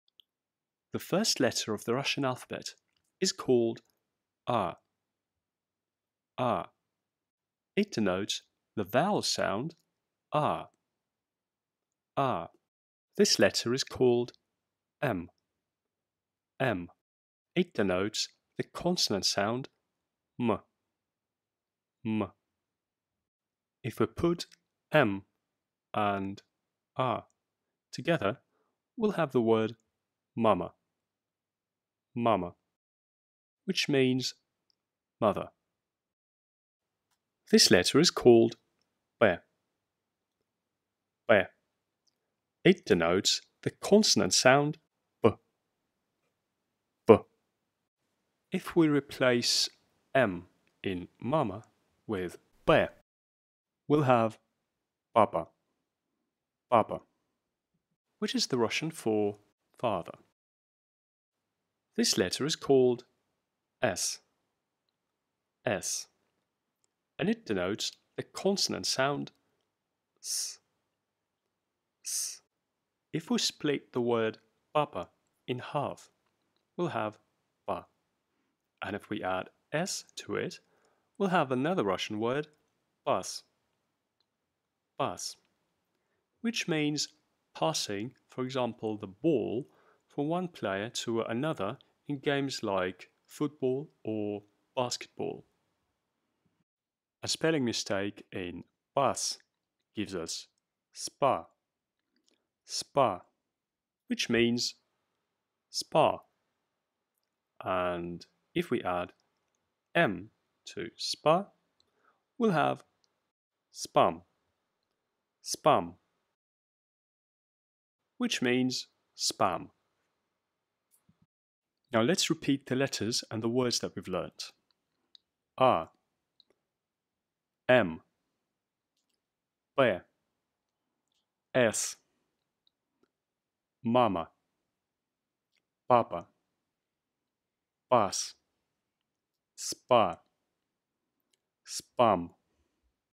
Russian Alphabet. Part 2. Letters А М П С
Learn the Russian alphabet with short easy-to-read words. Video for beginners of Russian.